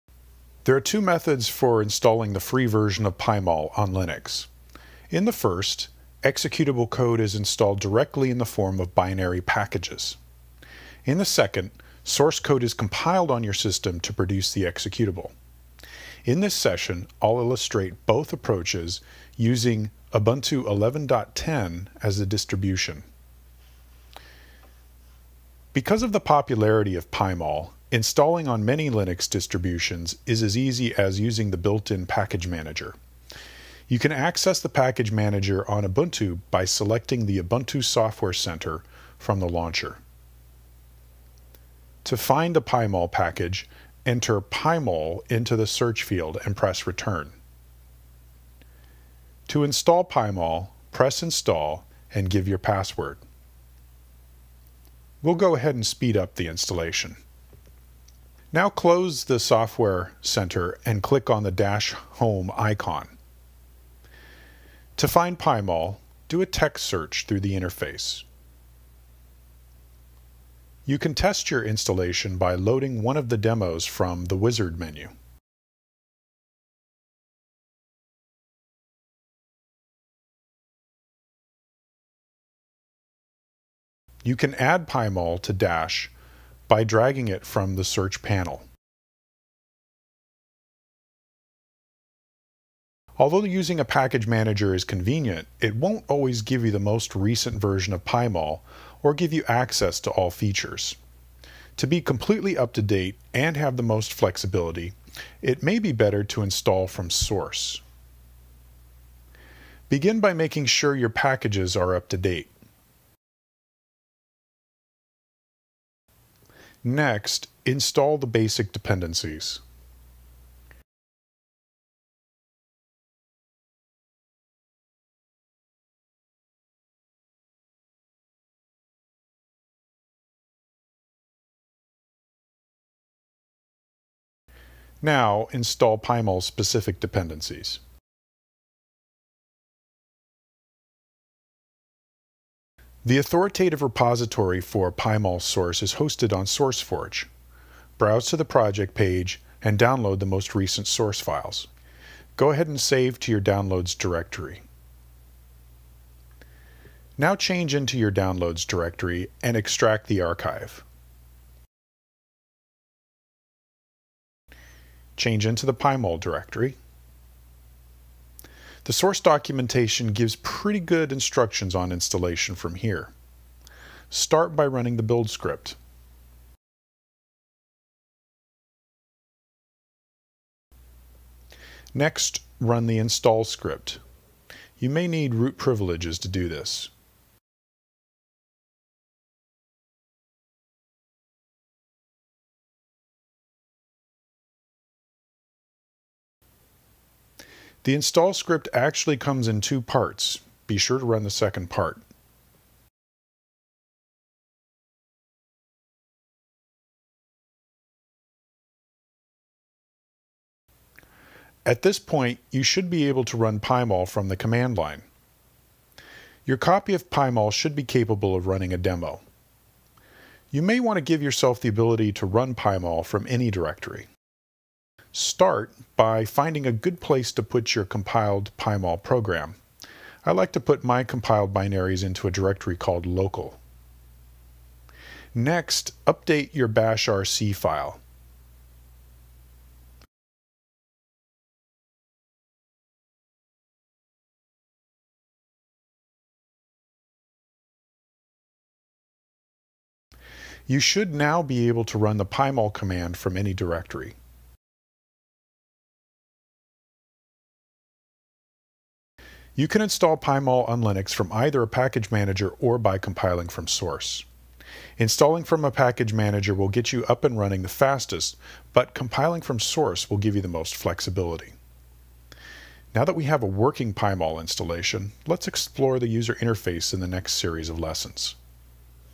Install PyMOL on Ubuntu Linux
for the full course.

PyMOL is an open source program for 3D molecular visualization. This tutorial shows how to install PyMOL on Ubuntu Linux through the package manager and by compiling source.